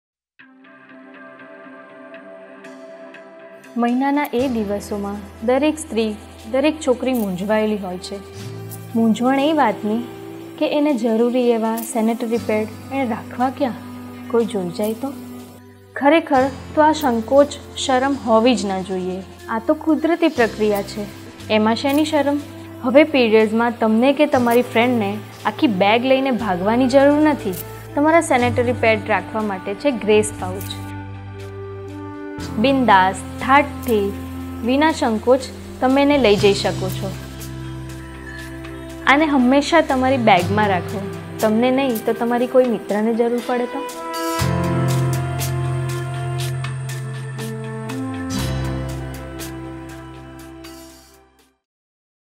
GRACE POUCH
Isnt it essential? Isnt it an ideal gift?
શું ગ્રેસ પાઉચ આજના સમયમાં ખુબજ જરૂરી નથી?
શું આ એક ઉમદા ભેટ નથી?

અમે માનીએ છે કે ગ્રેસ પાઉચ દરેક માનુની માટે ખુબજ જરૂરી છે.
સેનેટરી પેડ જેવી અતિ આવશ્યક ચીજ પર્સ માં કે બેગમાં ખુલ્લા રાખી શકાતા નથી, એનું કારણ શરમતો છેજ સાથે હાઇજીન પણ એટલોજ મહત્વનો મુદ્દો છે.
તેવા સમયે કુદરતી રંગોથી છપાયેલા અને હાથ વીણાટથી તૈયાર થયેલા.
સ્ત્રીઓ માટે, સ્ત્રીઓ દ્વારા ડીઝાઇન થયેલા, સ્ત્રીઓં દ્વારા સીવાયેલા આ રંગબેરંગી પાઉચ   એક અભિગમ છે.
અભિગમ છે આવા ખુબજ નાના લાગતા પણ ખુબજ મજબુત પગલે  ચેતના લાવવાનો.

વૈશાલી
ટીમ વેશ